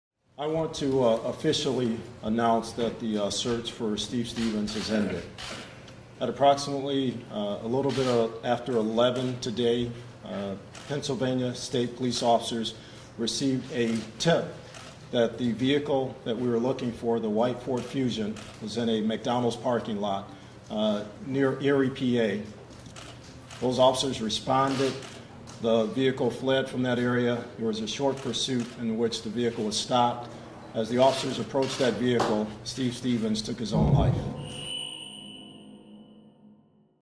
Search for Steve Stephens ends in Erie, Pennsylvania after police chase
Suspected killer Steve Stephens killed himself after a short chaise by police in Erie, Pennsylvania.  Video by David Petkiewicz, cleveland.com Photo by Greg Wohlford/Erie Times-News